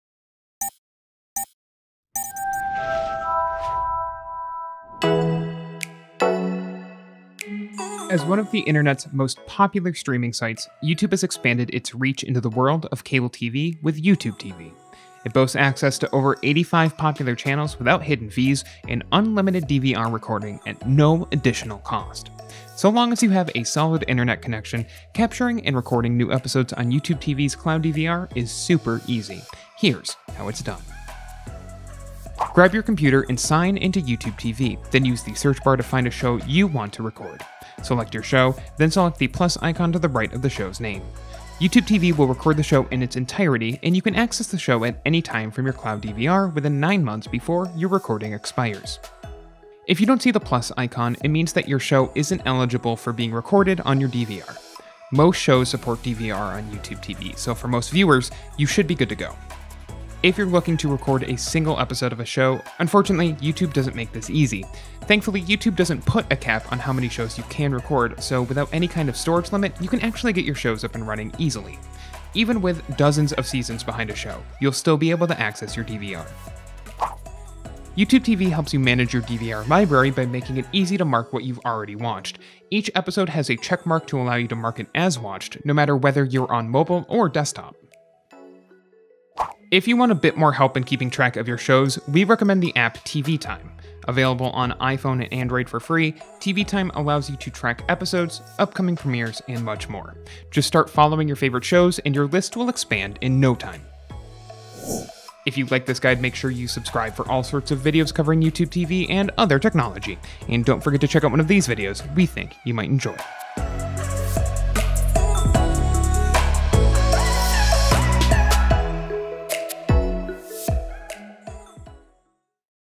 How to Only Record New Episodes in YouTube TV
As one of the internet's most popular streaming sites, YouTube has expanded its reach into the world of cable TV with YouTubeTV. It boasts access to over 85 popular channels without hidden fees and unlimited DVR recording at no additional cost. So long as you have a solid internet connection, capturing and recording new episodes on YouTube TV's cloud DVR is super easy. Here's how it's done!